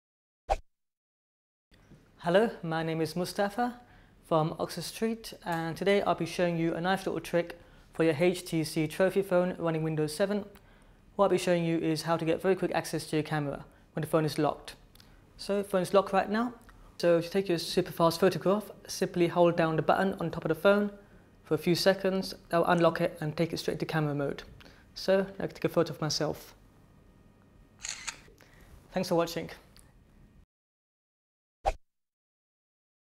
HTC Trophy: Quick camera access tip
By Mustafa Rashid
How to quickly access your camera on your HTC Windows 7 Trophy, for when your out and about and you quickly want to grab that shot